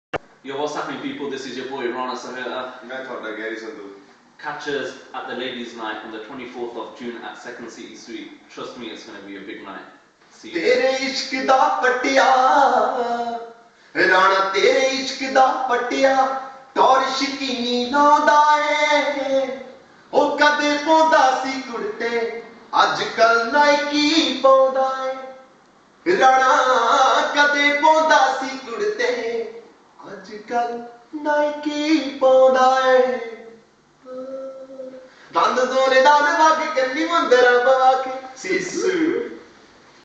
Summer Ladies Night 24th June 2011
SPICE MEDIA PRESENTS THE SUMMER LADIES NIGHT ON FRIDAY THE 24TH OF JUNE 2011 AT SECOND CITY SUITE, BIRMINGHAM.

PERFORMING ON THE NIGHT:

* GARRY SANDHU *
* FOJI *
* RANA SAHOTA *
* Dj H *
* JAGS KLIMAX *

AND MORE ON THE NIGHT

THREE COURSE MEAL PROVIDED BY SPICE CORNER (CATERING FOR VEG AND NON VEG)

ALSO INCLUDED ON THE NIGHT:

* FASHION BOUTIQUE *
* PROFESSIONAL MEHNDI ARTIST *
* PROFESSIONAL PHOTOGRAPHY *

PLUS MORE

DRESS CODE: TRADITIONAL/EVENING WEAR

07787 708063